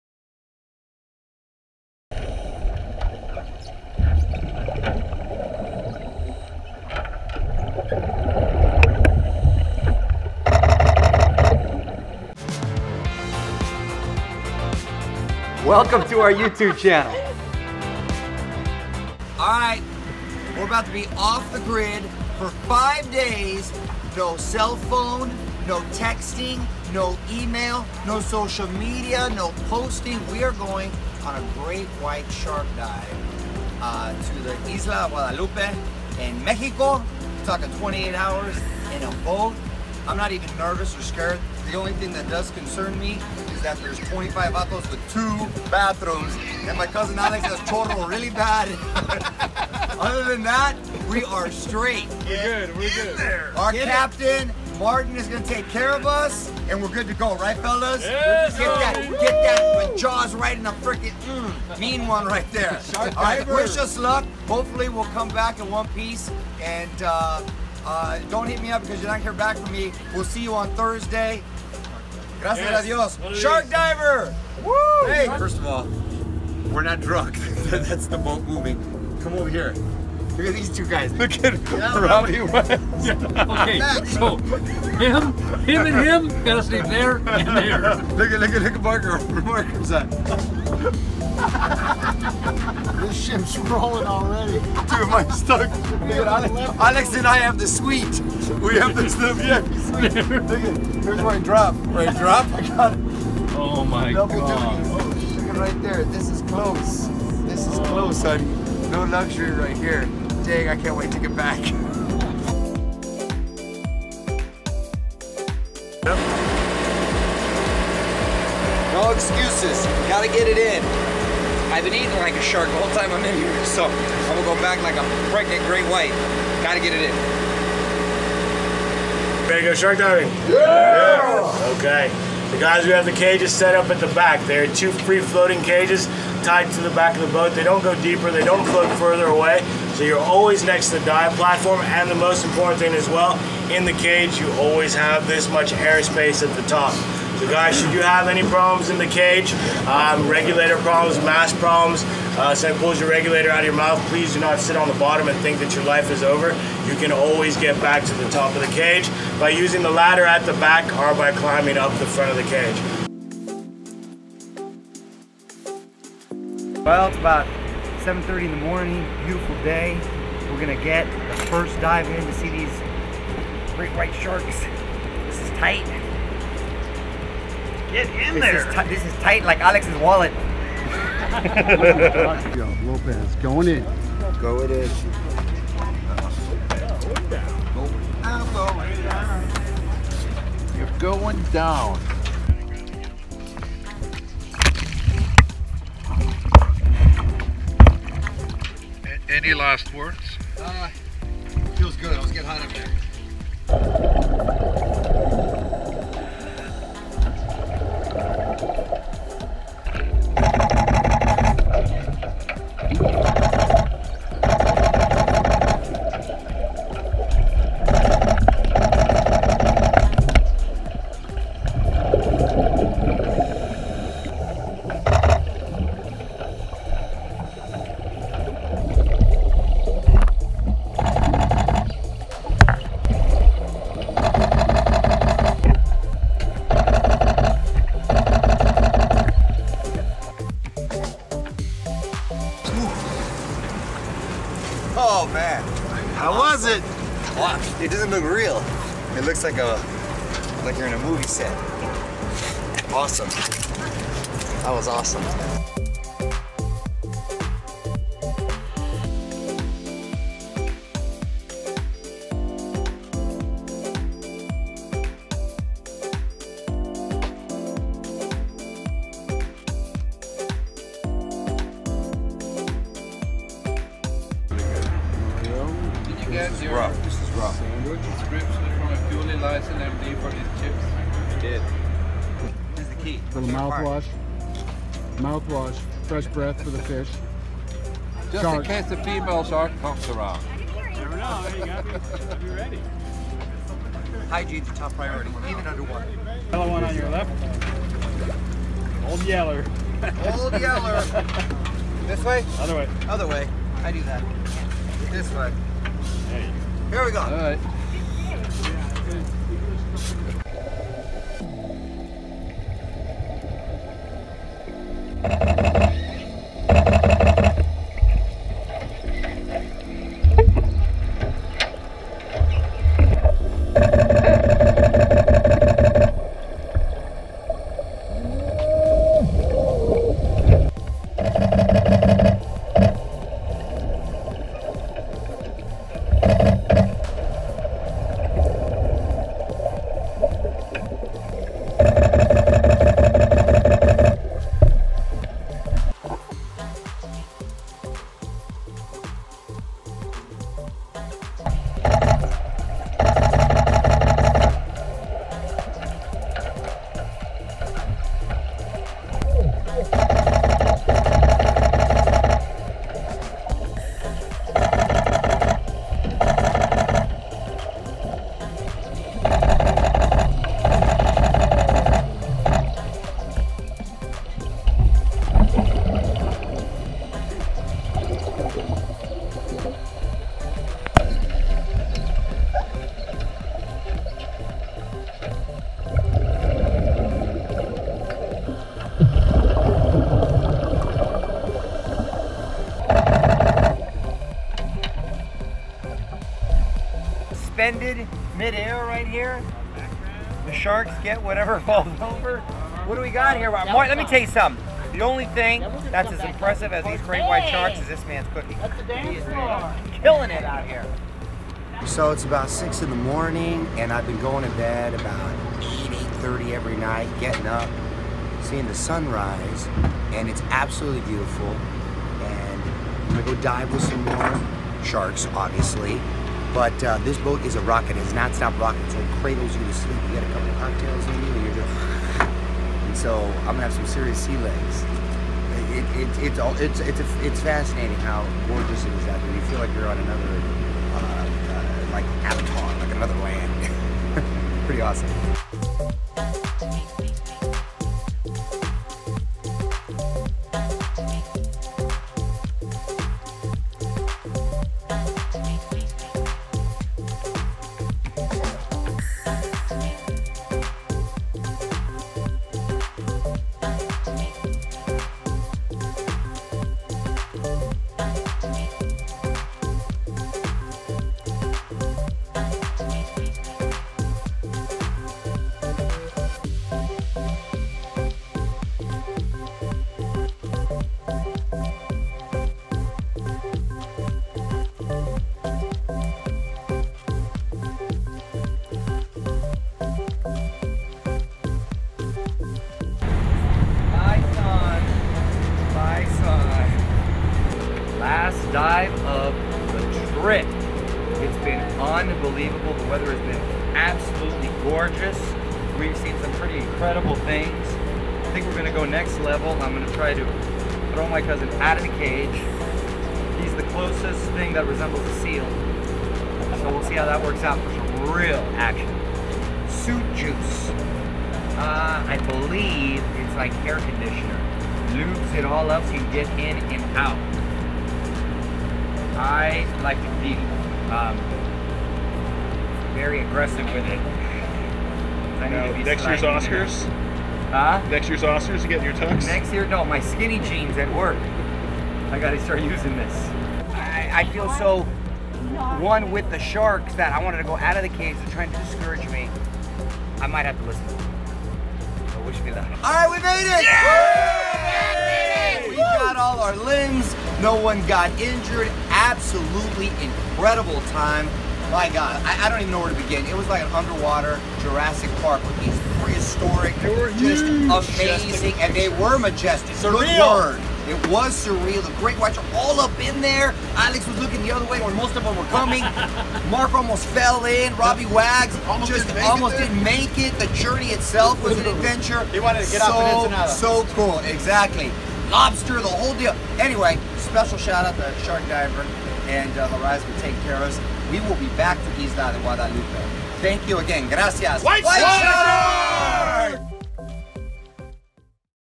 Great White Shark Diving with Mario Lopez
Mario went shark diving! He spent 5 days off the grid on Guadalupe Island in Mexico and vlogged the experience to share with you guys. Would you go shark diving?